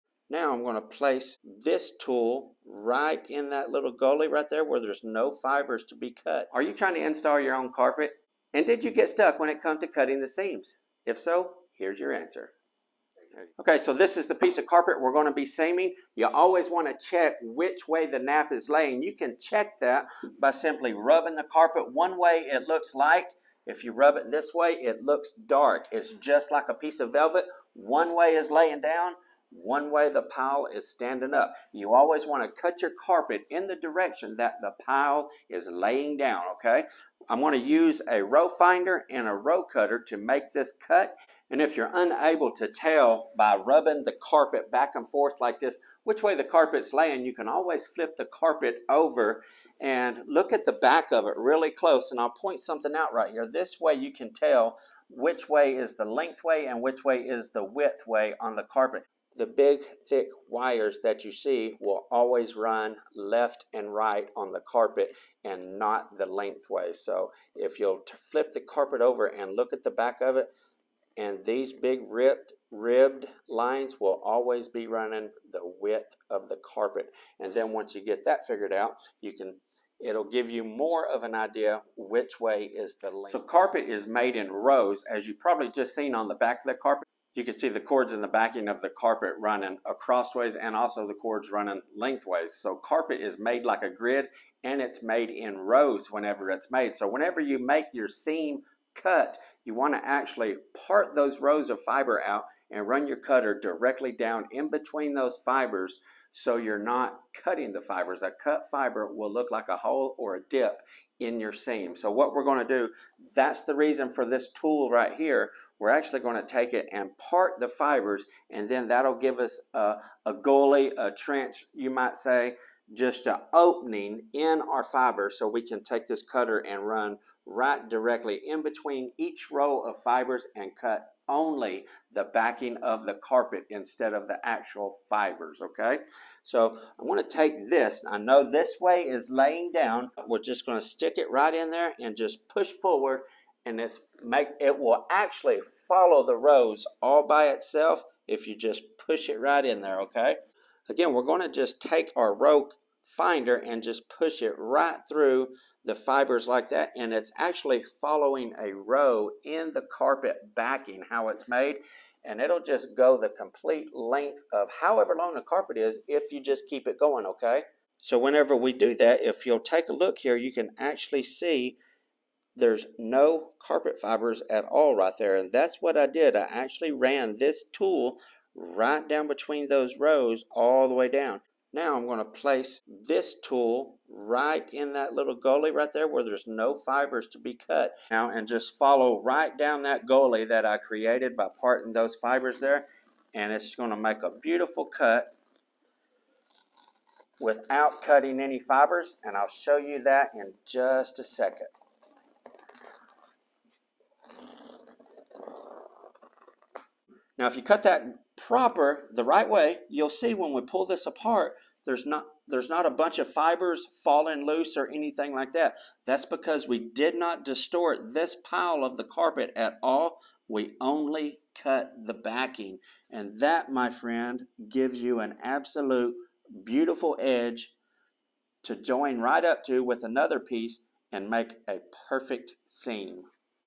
How You Cut A Carpet Seam
In this video, I give you super simple, step-by-step instructions how you can cut a Carpet Seam